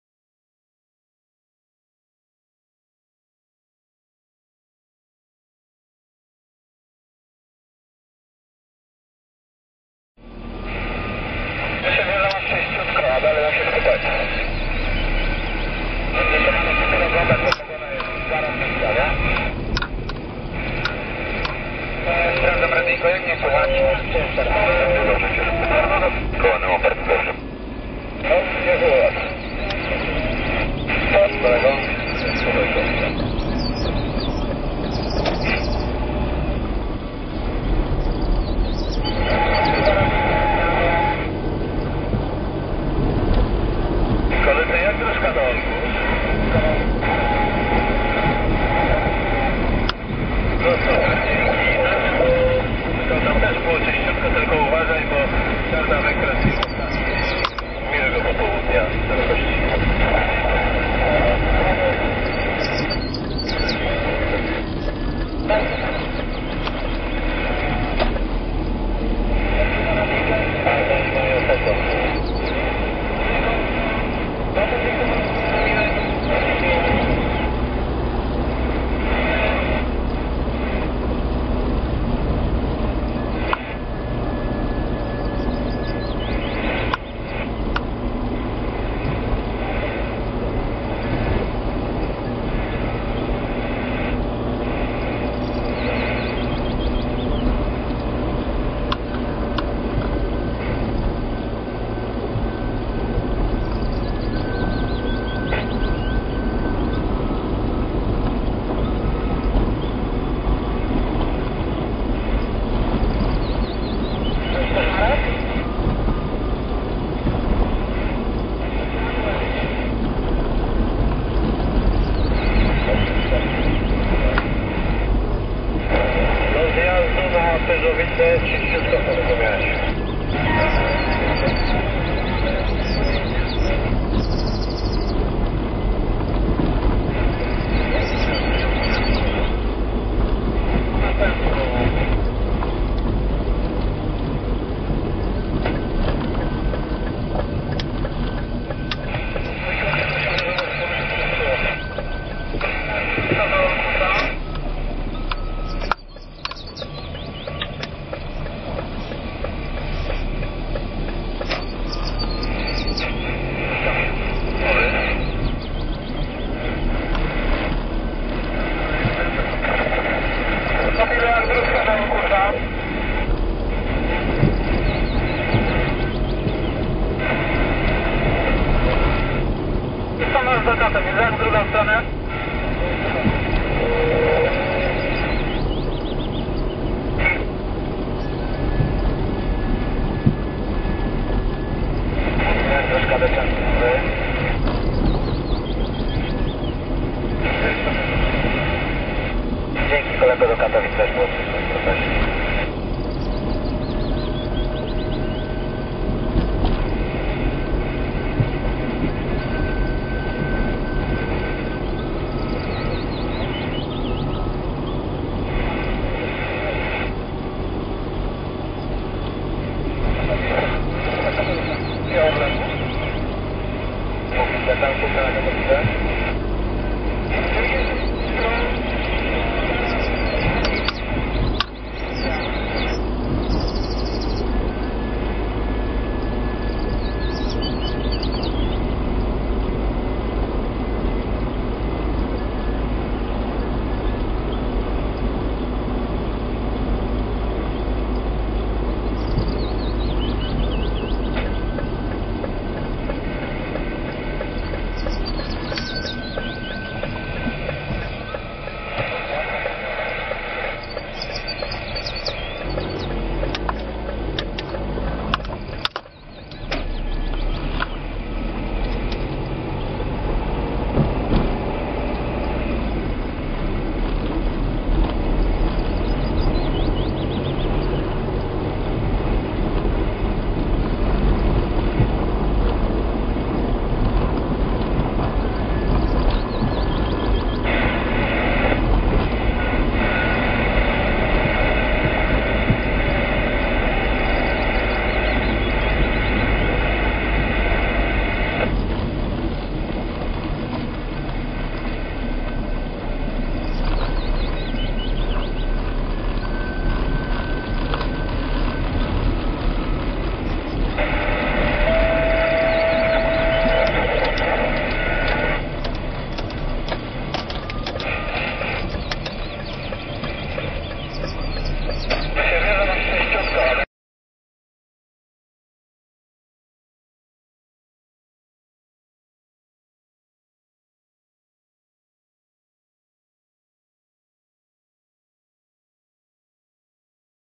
20080309 - Dąbrowa Górnicza. Przejazd ul. Letnią na Zielonej
20080309_04b - Dąbrowa Górnicza. Przejazd ulicą Letnią w Parku "Zielona".